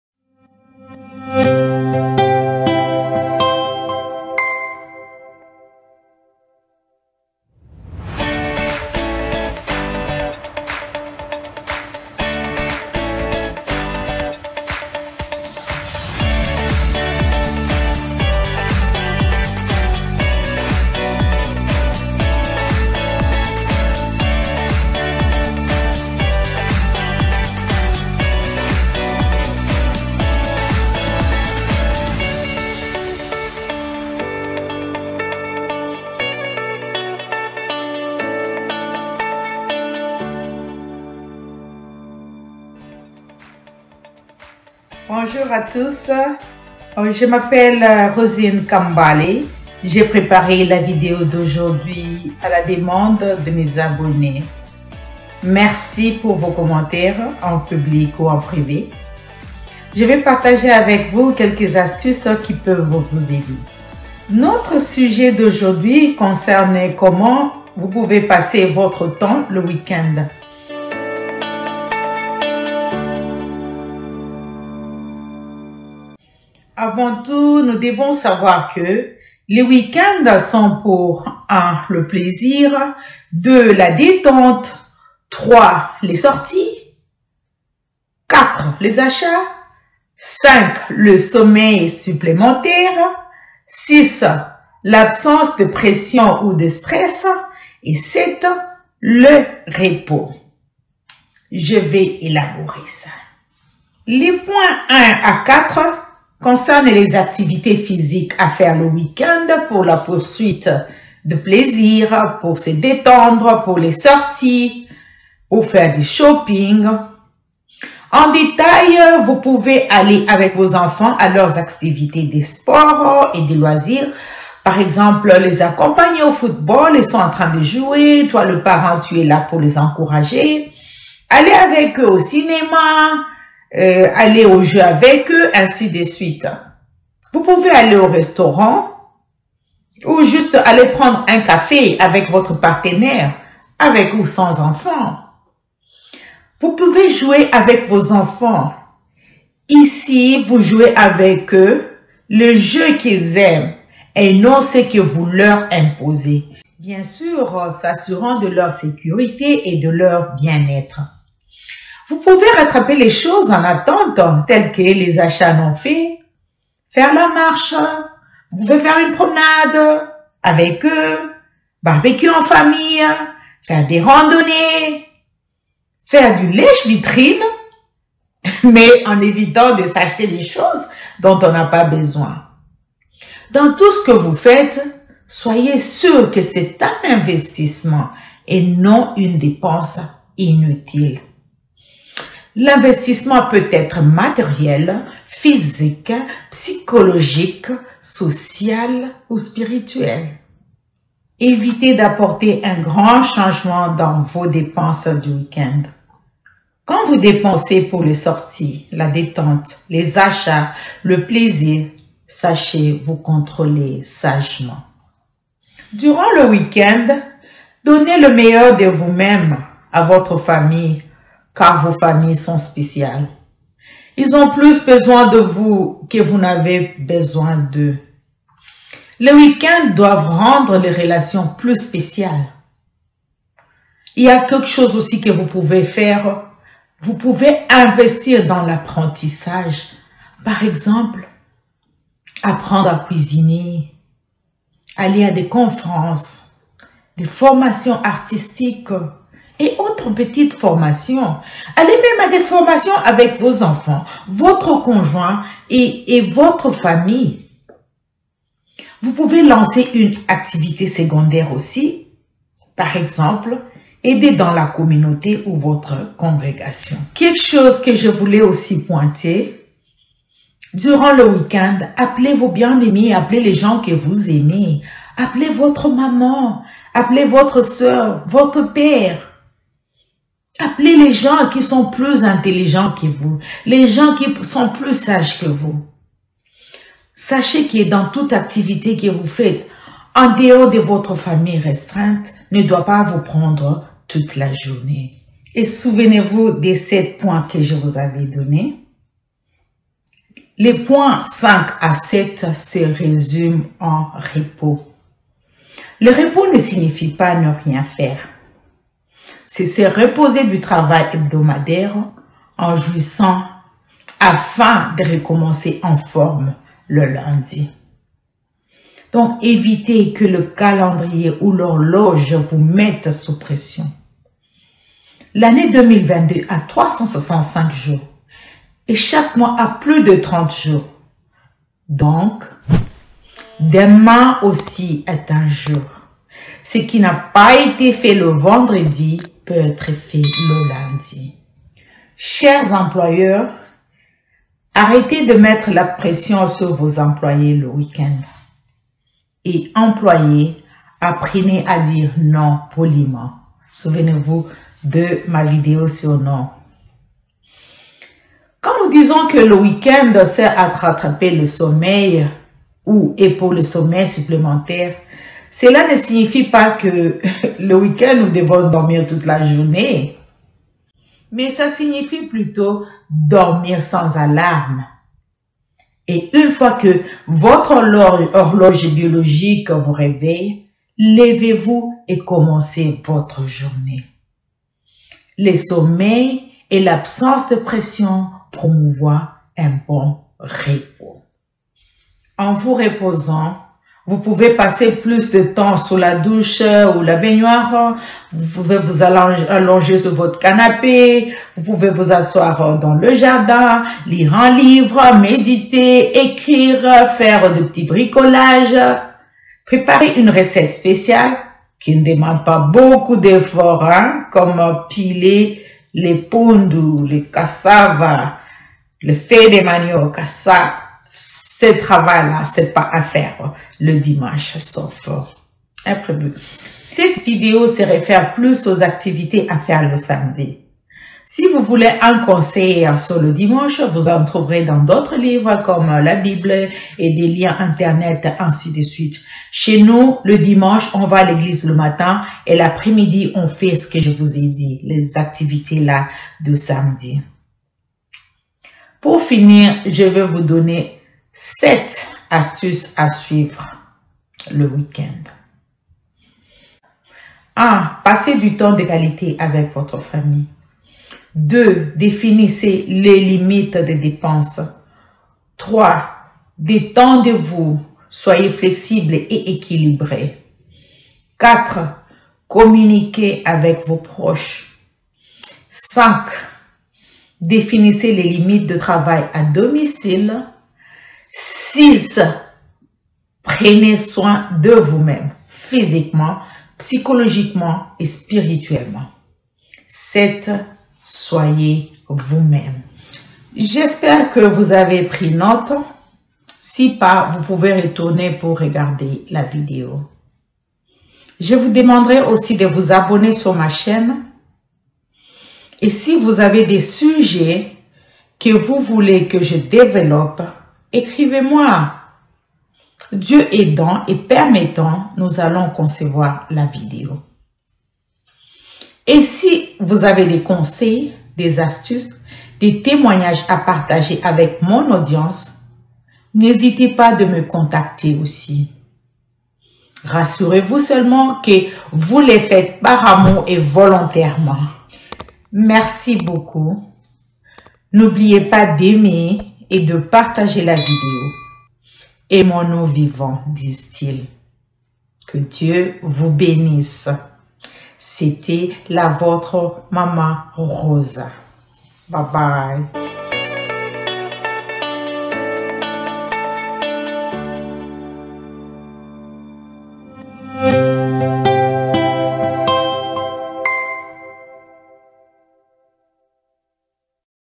Comment planifier un weekend rentable
Mama Rosa explique a quoi sert le weekend puis elle donne des conseils pour bien profiter du week-end:
les astuces de Mama Rosa